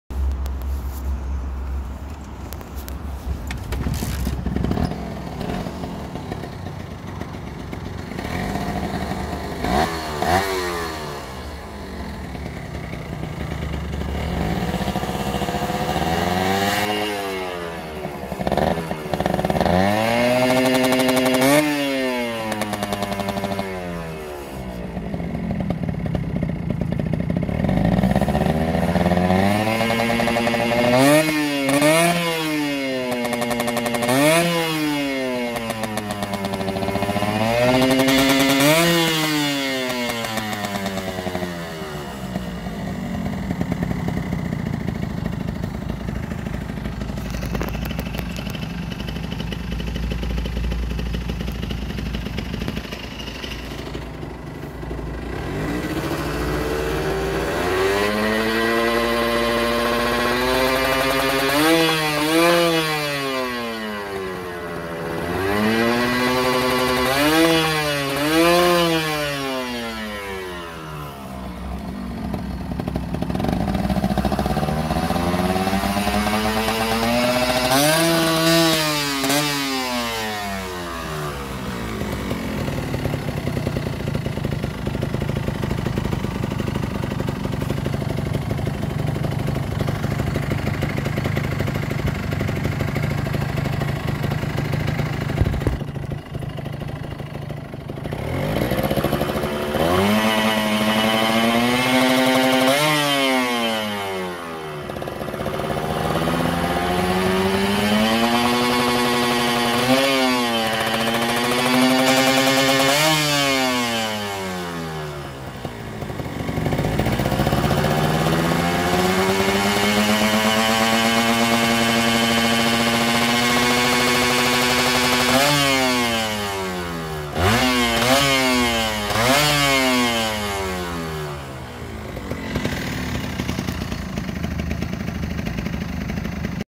Ninja R 150 | Suara mesin & knalpot standard
Test suara Knalpot ninja R 150 & superkips test

kondisi mesin & knalpot bawaan pabrik